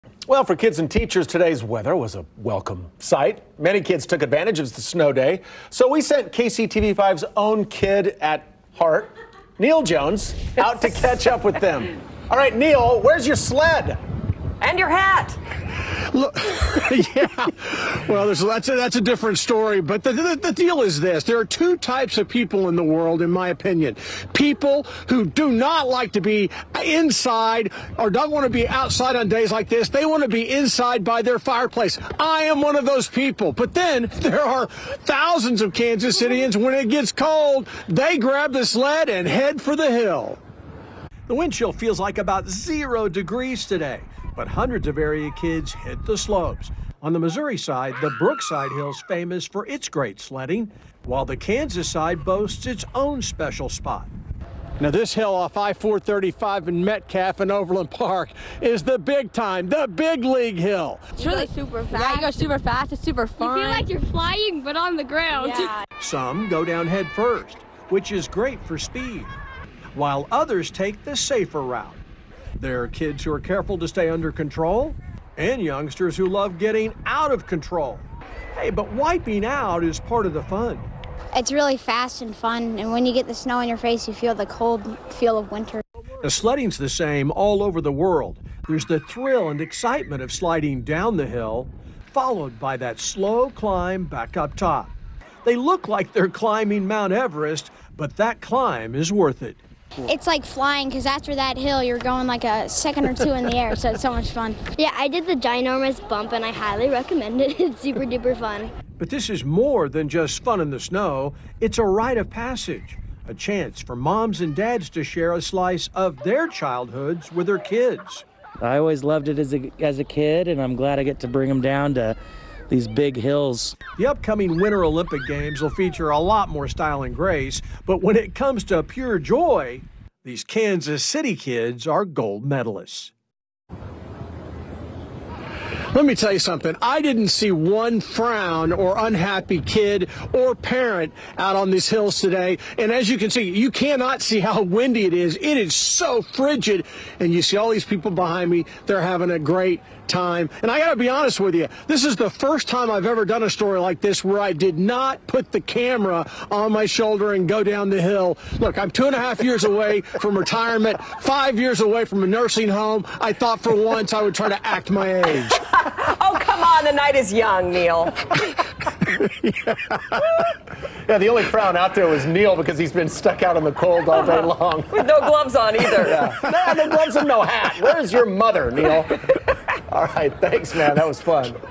Local children enjoy snow day, go sledding
For kids and teachers, today's weather was a welcome sight. Many kids took advantage of the snow day. So, we sent KCTV5's own kid (at heart) Neal Jones out to catch up with them!